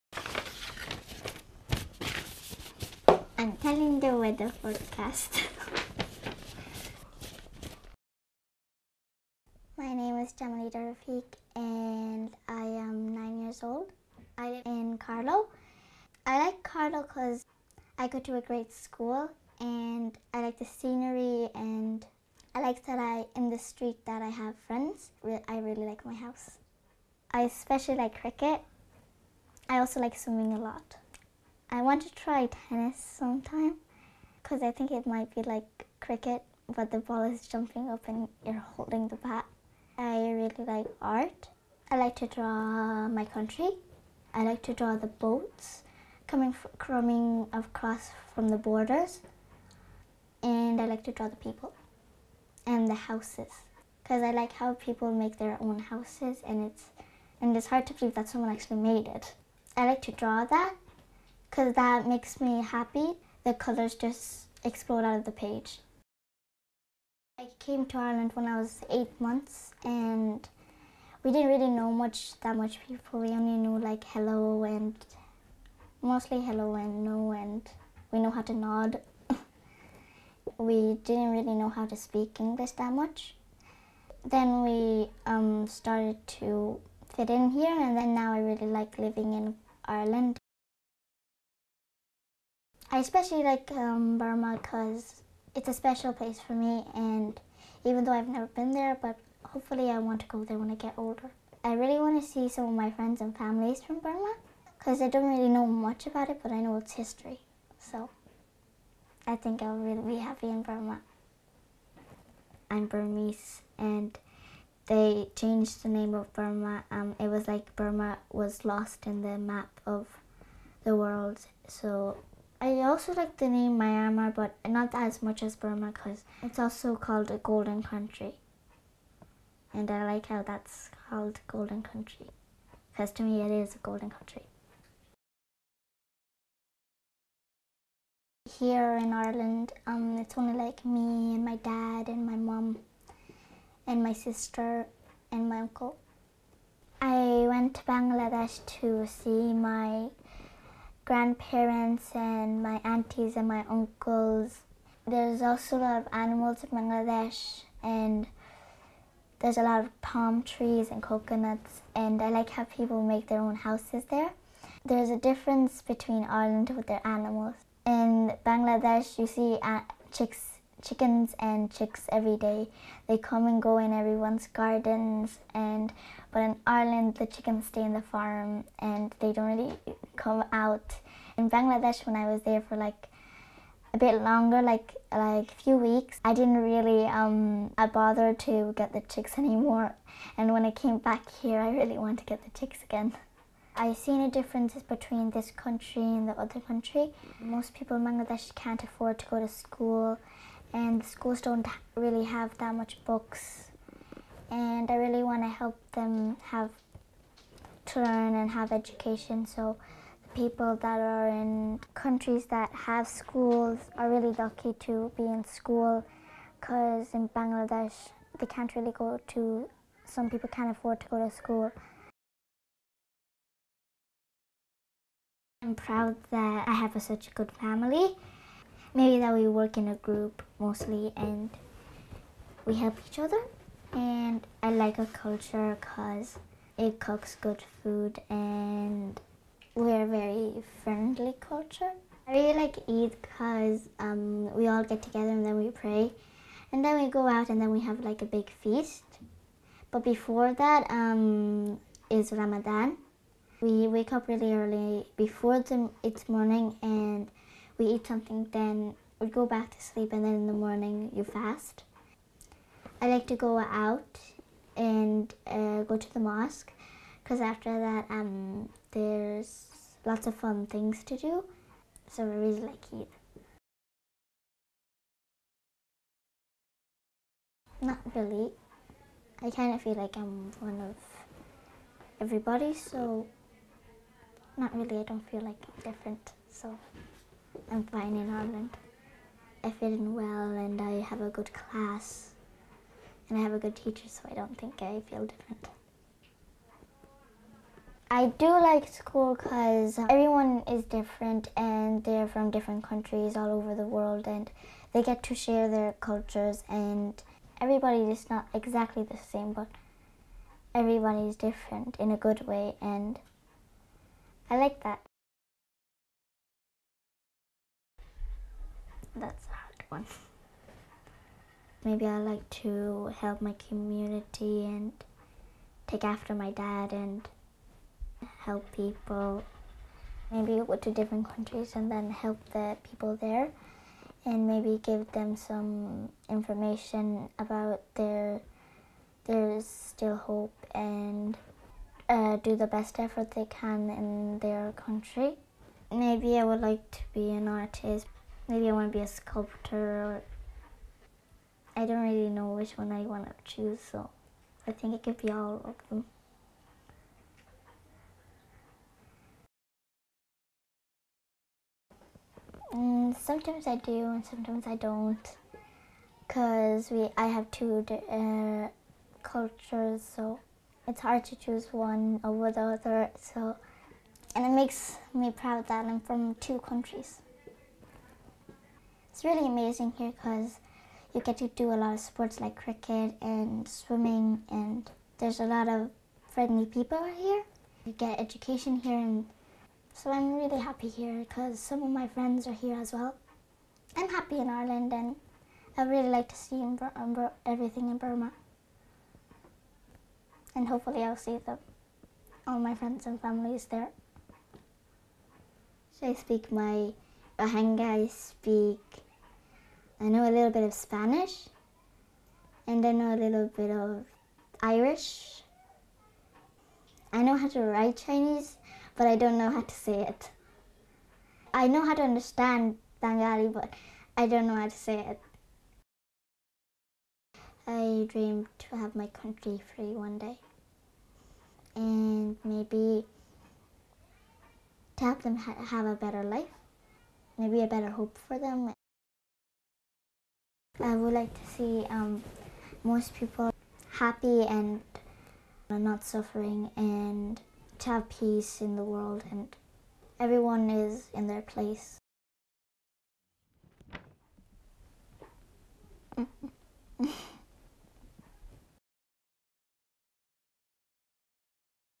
Uprooted - An interview with Jamalida from Carlow
Jamalida (9) lives in Carlow. She is Burmese, but has never been to Burma (Myanmar).  One of her favourite pastimes is drawing pictures of Burma: “I like to draw the boats coming across from the border. I especially like to draw the people.” Jamalida was born in a refugee camp in Bangladesh and travelled to Ireland as a baby.

Jamalida loves living in Ireland, but she says it was hard to fit in at first. Learning English helped. She says: “Now I have two cultures, and it’s hard to pick one over the other.”

Finding a place to fit in is important to her: “I would like to see everyone in their place” she says.

When she grows up, Jamalida isn’t exactly sure what she would like to be but she has big ideas. “I dream to have my country free one day. I would like to give my community some information about how to have hope, how to do the best with their lives.”

Uprooted is a UNICEF campaign centred around children living in Ireland who are refugees and migrants. This campaign aims to raise awareness and knowledge of children's rights.